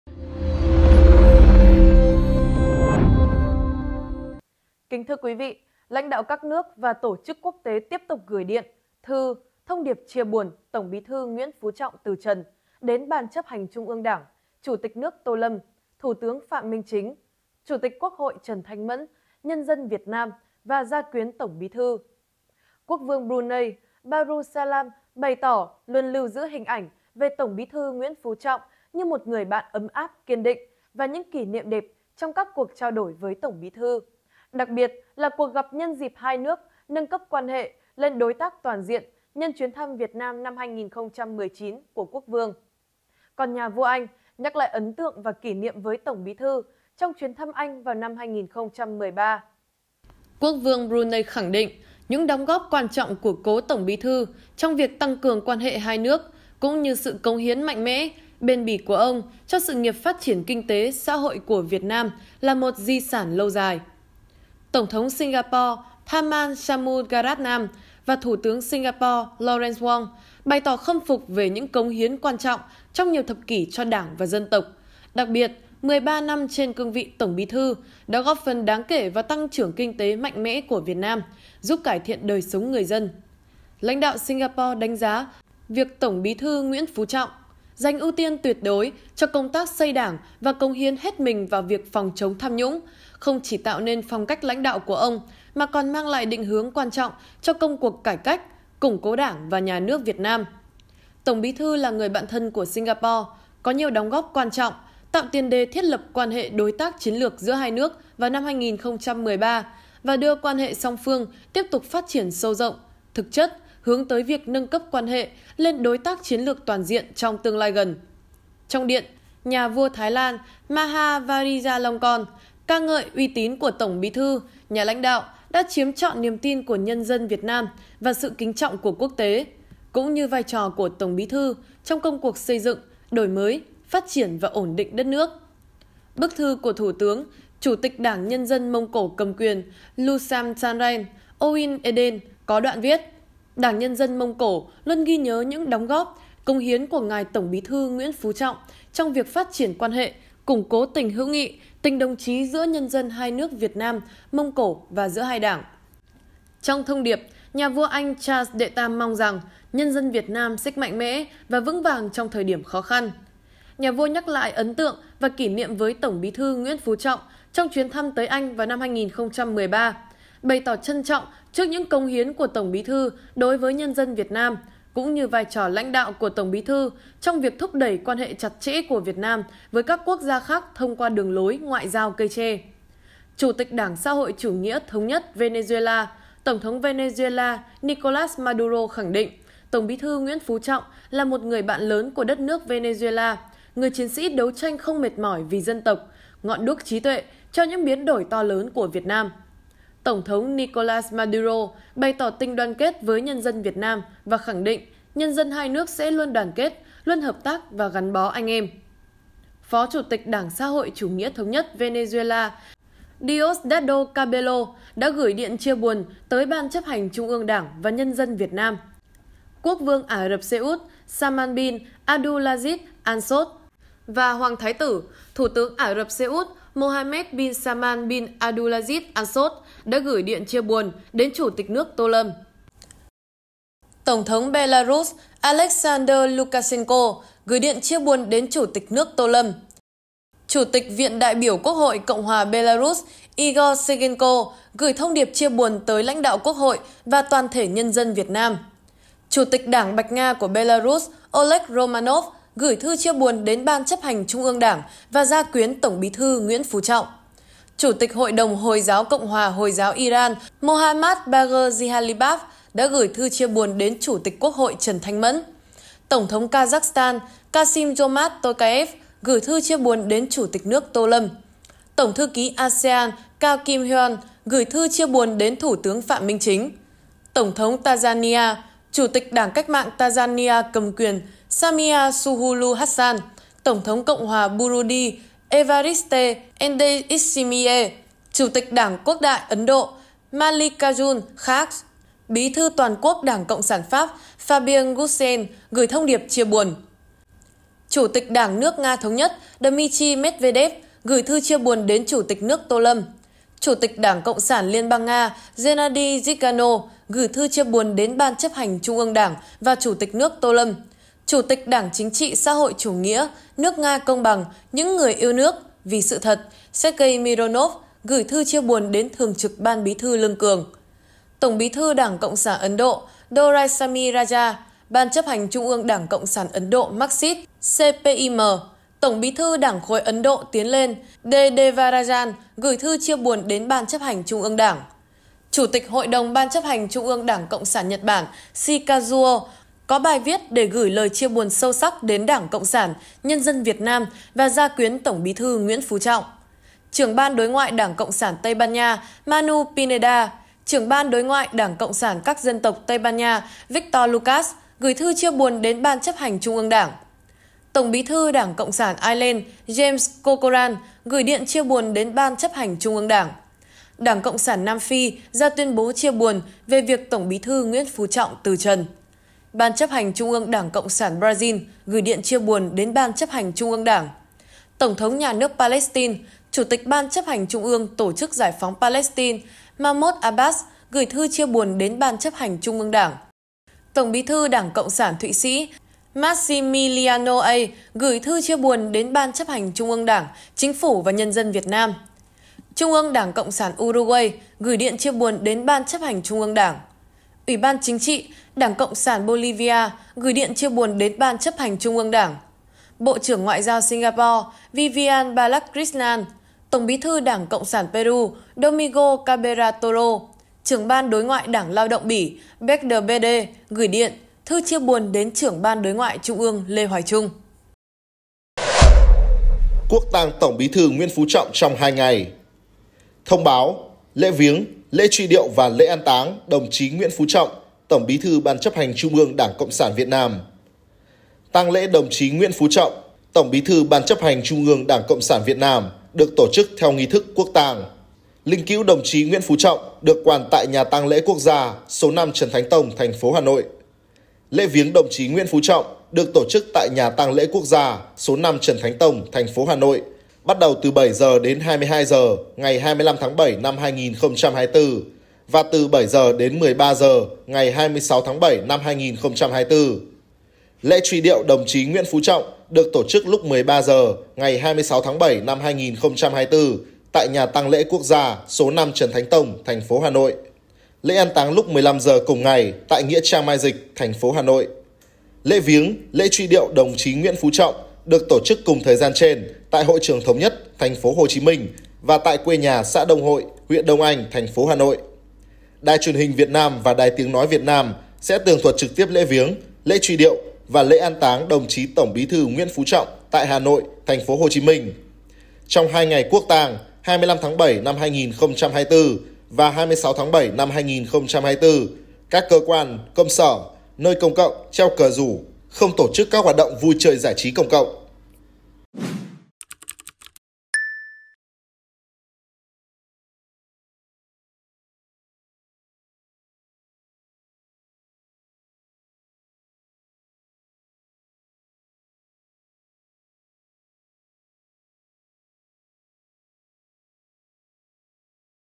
Nhà vua Anh, Quốc vương Brunei nhắc lại kỷ niệm về Tổng Bí thư Nguyễn Phú Trọng | Báo VietNamNet
Quốc vương Brunei Darussalam bày tỏ luôn lưu giữ hình ảnh về Tổng Bí thư Nguyễn Phú Trọng như một người bạn ấm áp. Còn Nhà vua Anh nhắc lại ấn tượng và kỷ niệm với Tổng Bí thư trong chuyến thăm Anh vào năm 2013.